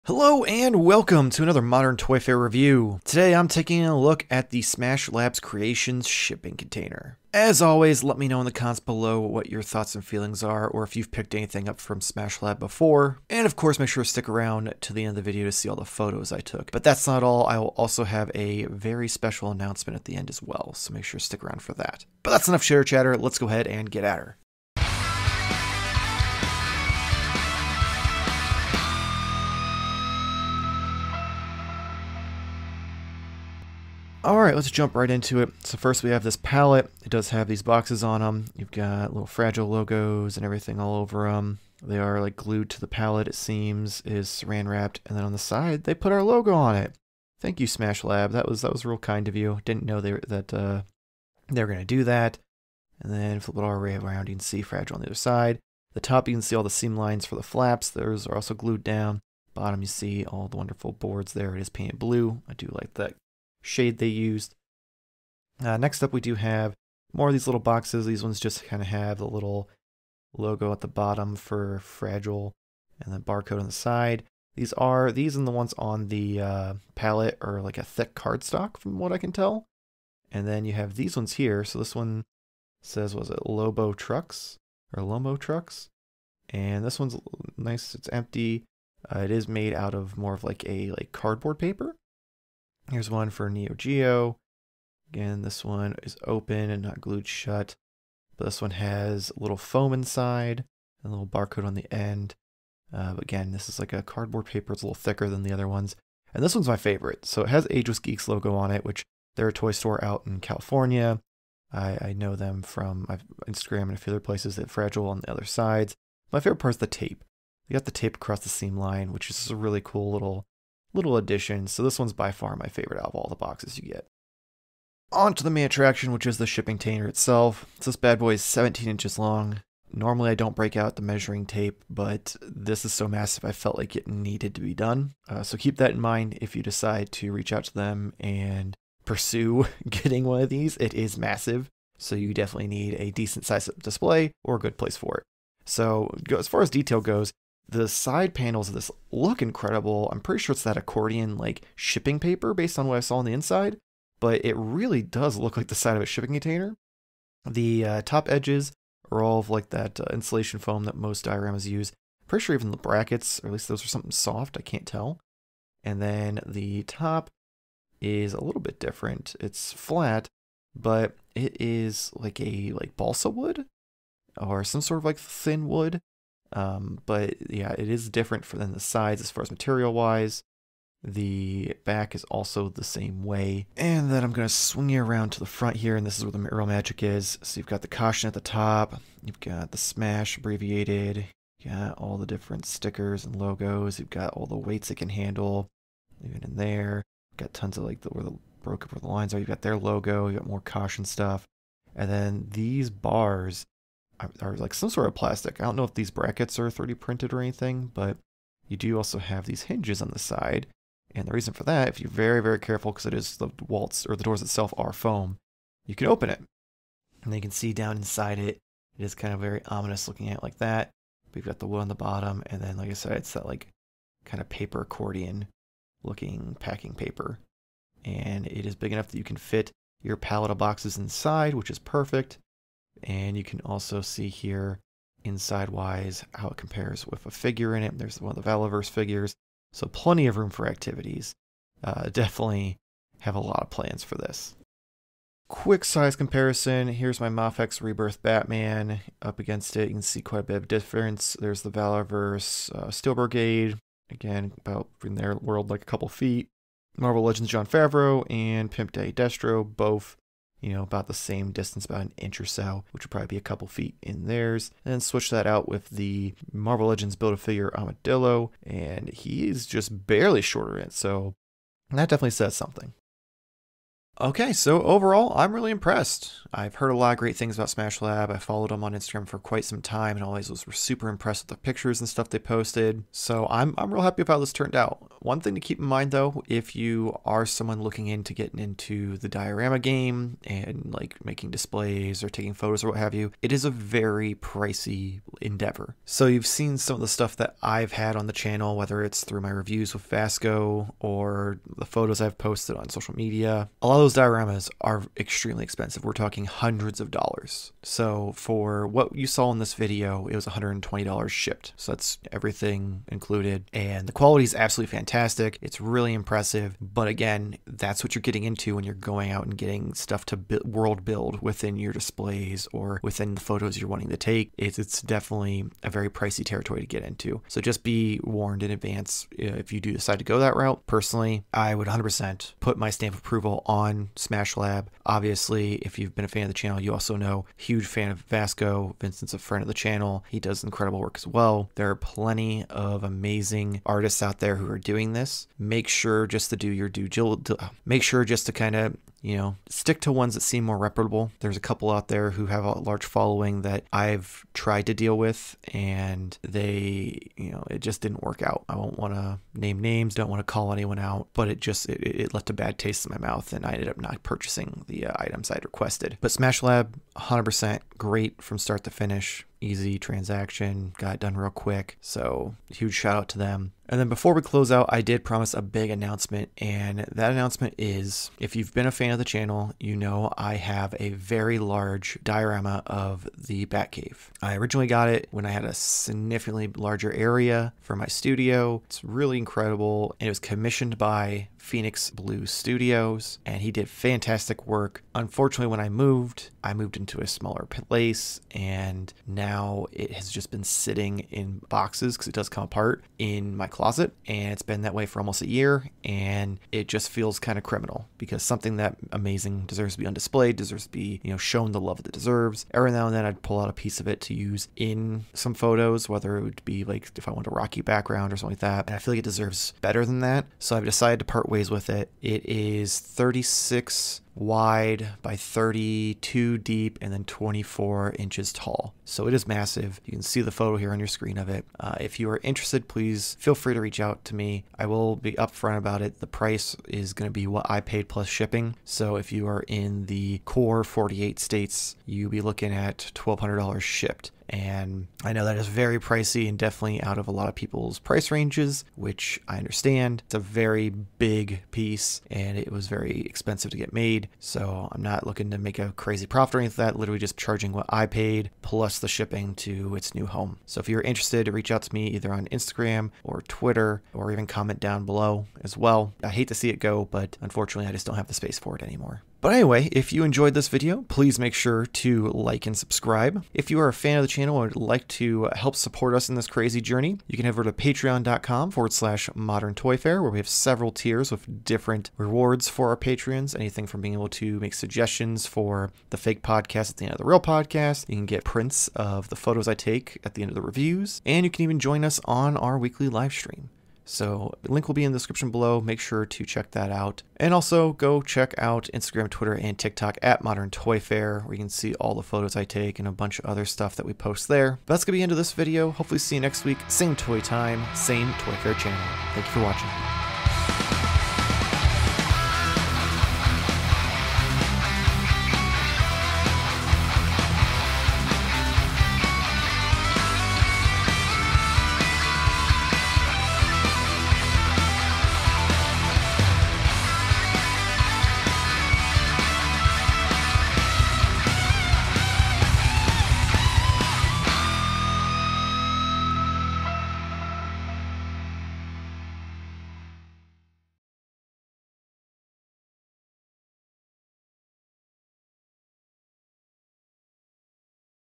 Smash Labs Creations Custom Diorama Shipping Container Review
Smash Labs Creations Custom Diorama Shipping Container Action Figure Review. I finally got my hands on the Smash Labs Creations Shipping Container and I'm excited to be taking a look at it.

Timestamps
0:00 Intro to Smash Labs Creations Custom Diorama Shipping Container Action figure Review
0:33 Accessories
2:05 Details and Sculpt
4:04 Size Comparison
4:34 Final Thoughts
8:32 Smash Labs Creations Shipping Container figure photography

It helps the channel which helps me get more reviews out for your viewing pleasure!

Check out Weekly Purchases every Wednesday on Twitch.tv/ModernToyfare

Music for intro by Daniel Figueroa
Diorama set by @VascoToys on instagram